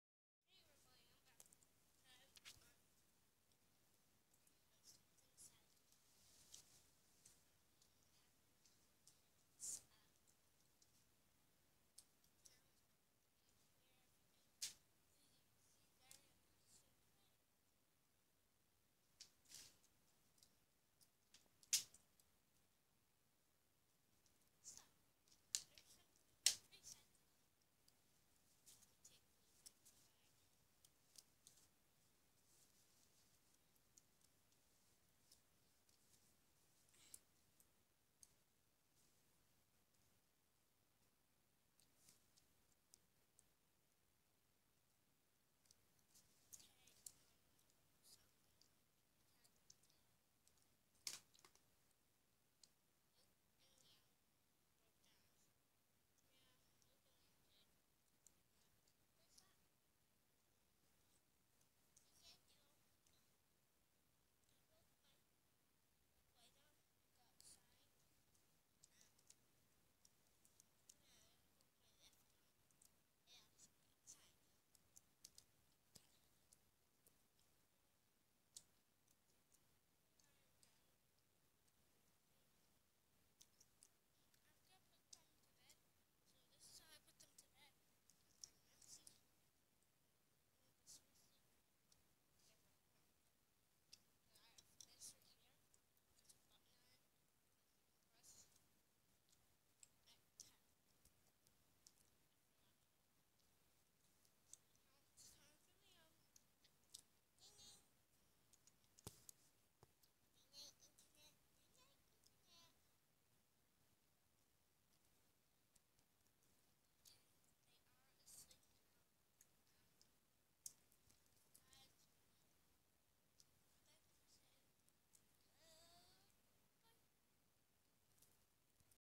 A Luke and Liam vid
Sad news everyone.Luke and Liam broke their arms😦 (well one arm.)
Also leave a friendly commet